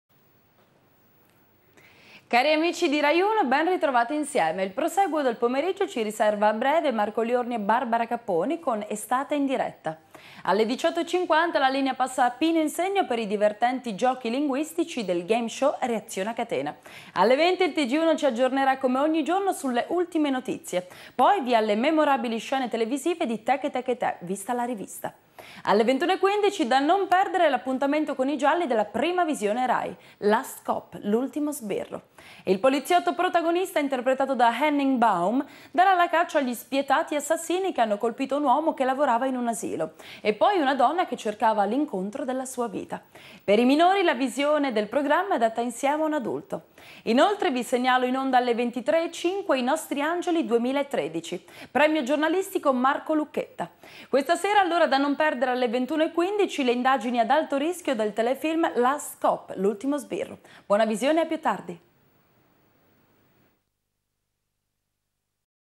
Raiuno - Annuncio - Luglio 2013
un annuncio pomeridiano andato in onda su raiuno nel luglio 2013...questa grafica è stata adottata dal 28 novembre 2011 ed è trasmessa tutt'oggi